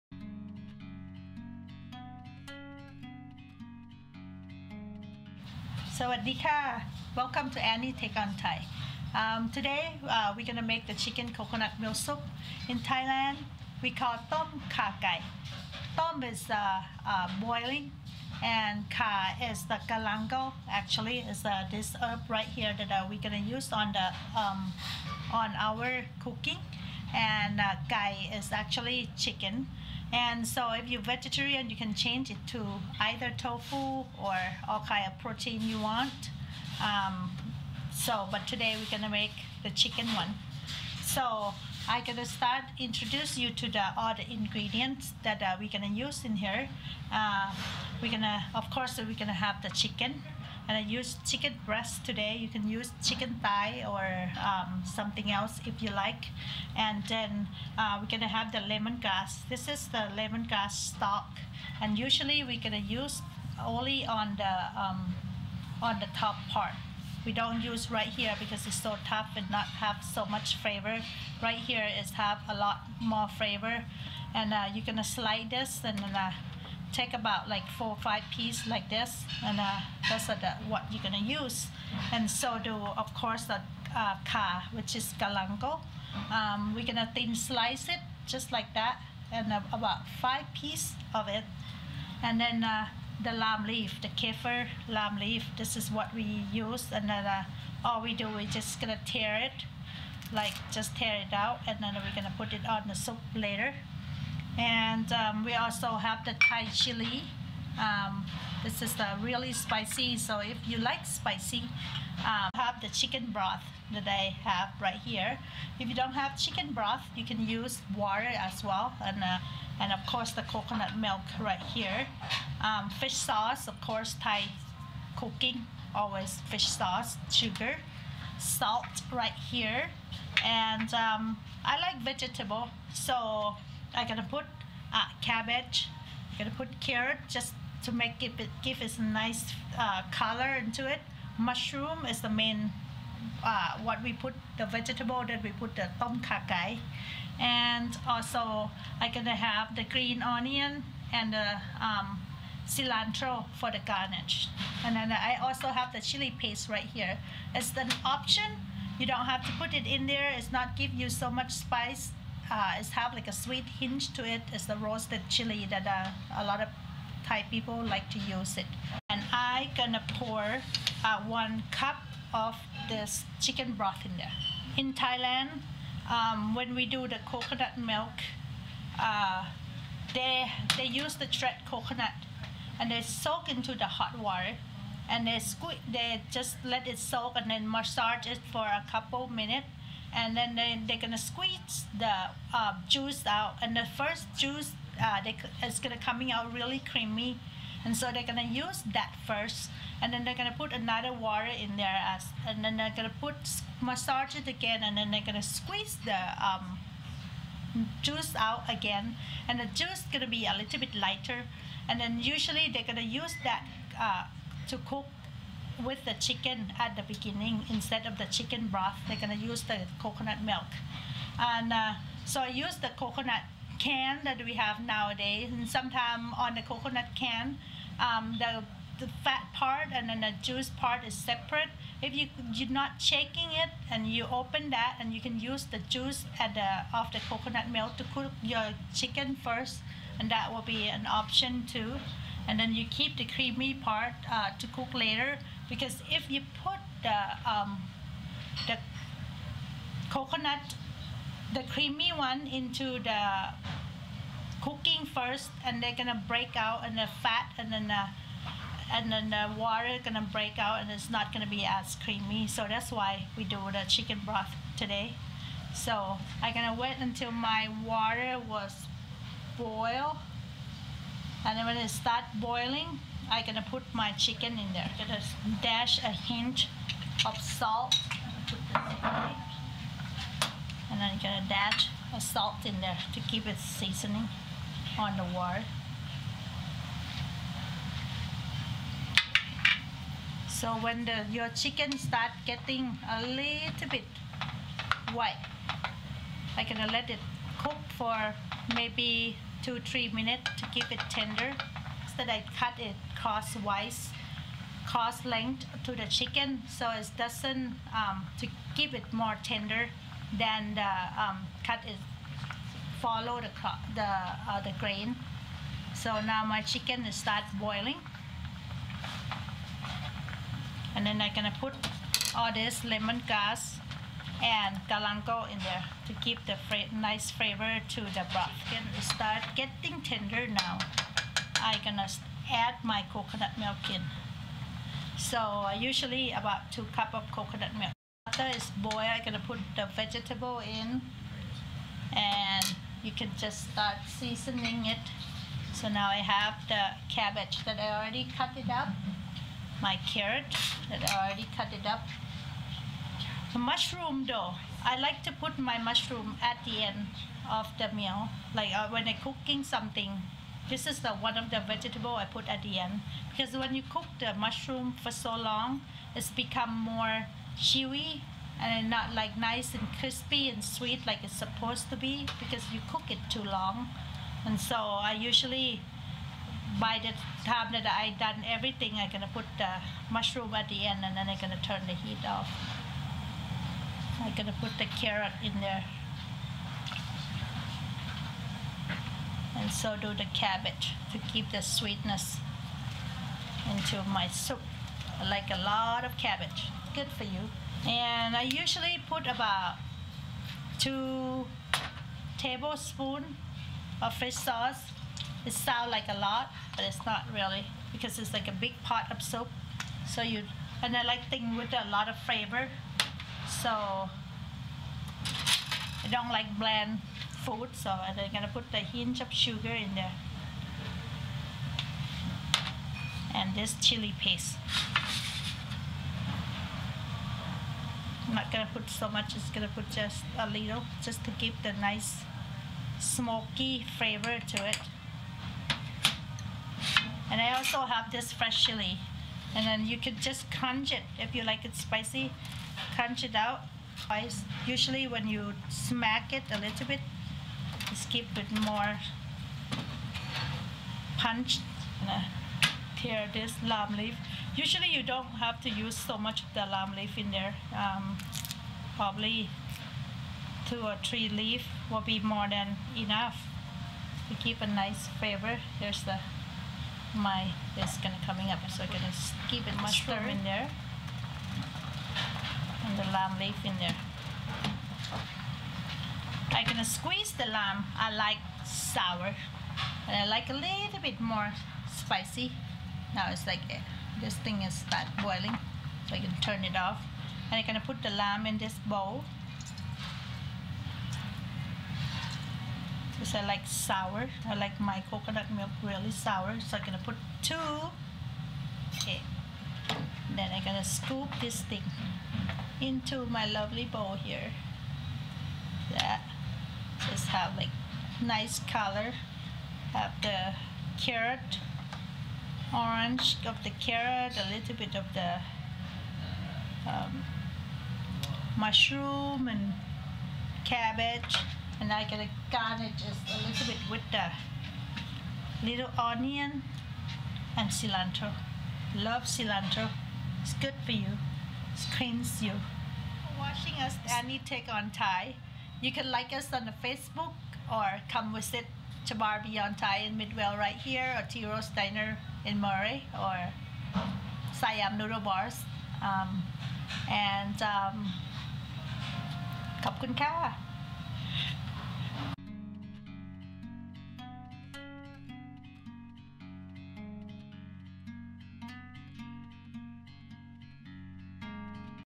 Learn How to Cook Thai Coconut Milk Soup
Anny Sooksri shows you how to make chicken coconut soup